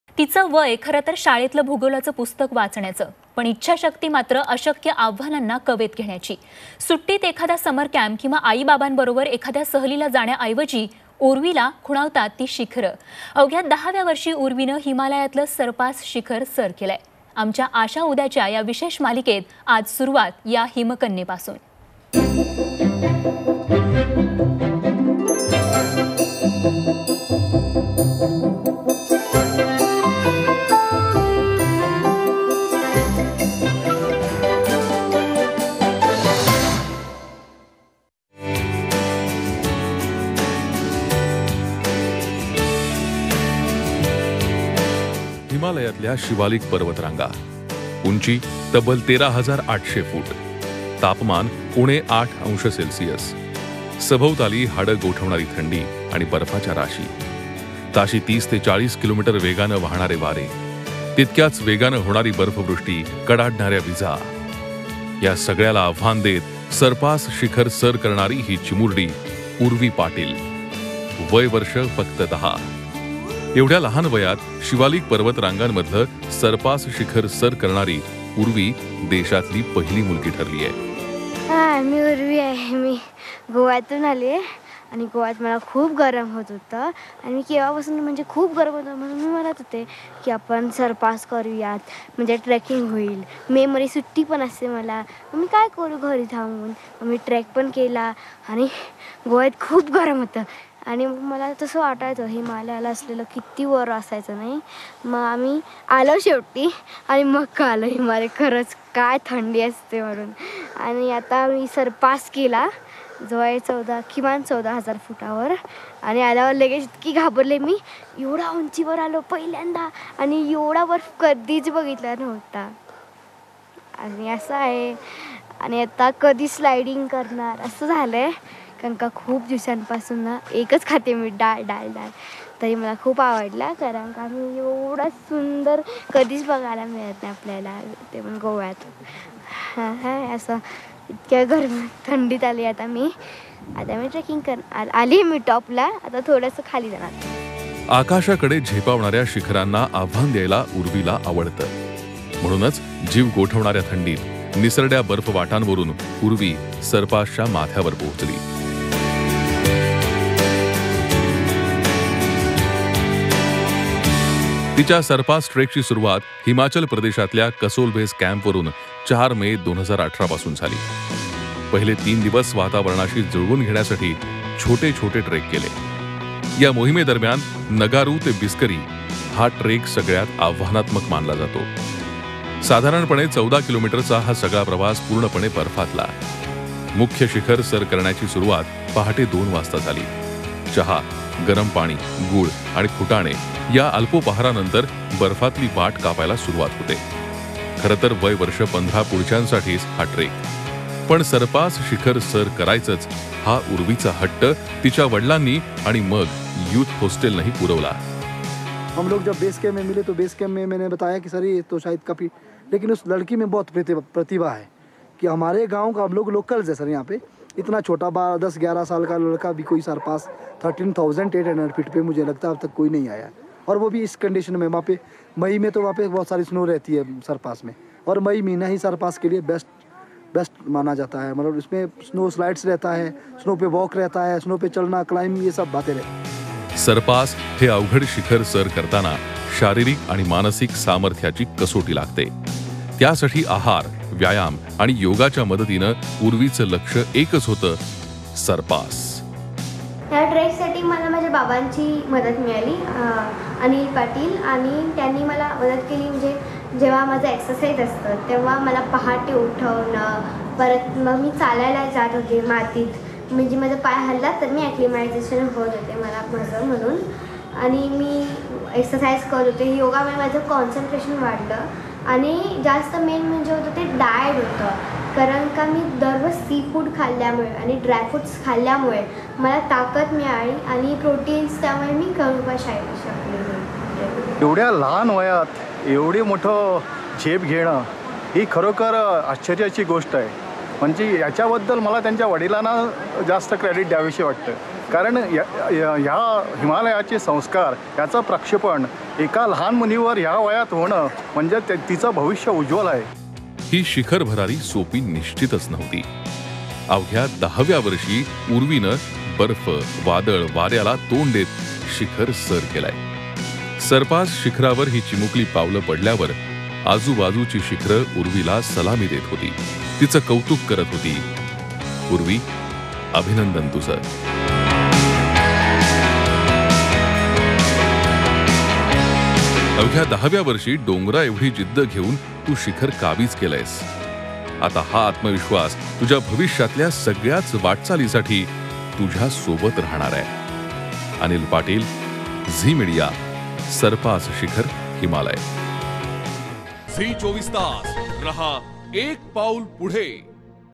गोवा | आपल्या घरातील लहान मुलांना हा व्हिडीओ नक्की दाखवा!
Goa | Youngest Trecker | urvi Patil To Climb Sar Pass Trek | Himalaya